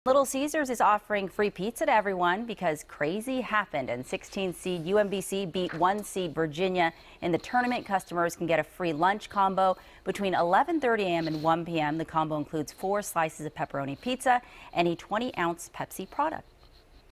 Little Caesars Offers Free Lunch Thanks To UMBC
You can thank the UMBC men's basketball team for lunch, as Little Caesars is offering free meals after their historic NCAA tournament win.